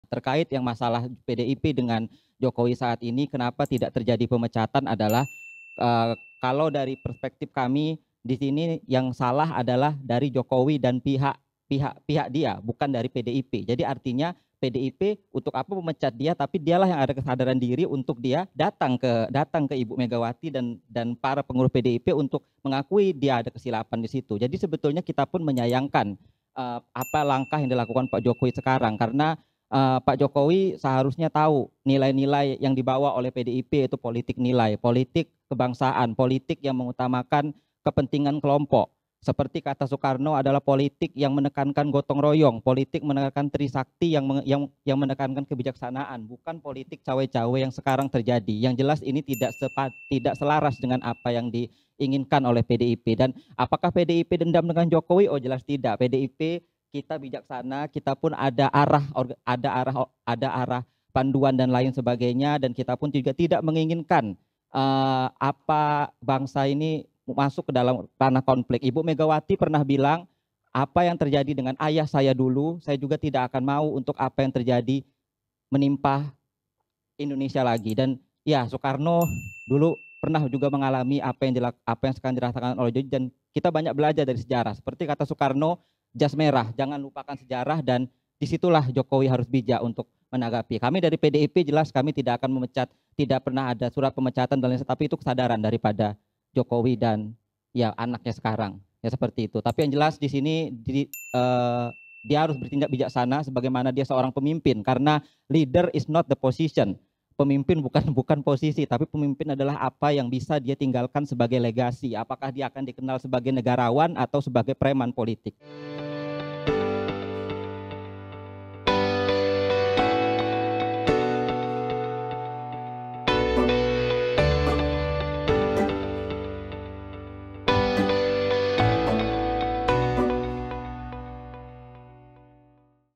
Jokowi perlu jumpa Ibu Megawati untuk akui kesilapan
Presiden Jokowi perlu berjumpa dengan pengerusi parti, Megawati Sukarnoputri dan mengakui kesilapan beliau yang tidak lagi menyokong Parti Demokrasi Indonesia Perjuangan (PDI-P).

Pengerusi Muda PDI-P Malaysia, Mansurni Abadi berkata, walaupun apa yang dilakukan sememangnya salah, namun pihaknya tidak pernah nafikan jasa Jokowi selama ini.

Parti itu bertindak secara bijaksana dalam isu ini dan memberi keutamaan kepada rakyat Indonesia.

Beliau berkata demikian dalam program BERANI SEMUKA: PILPRES 2024 INDONESIA PILIH SIAPA? di semua platform digital Sinar Harian.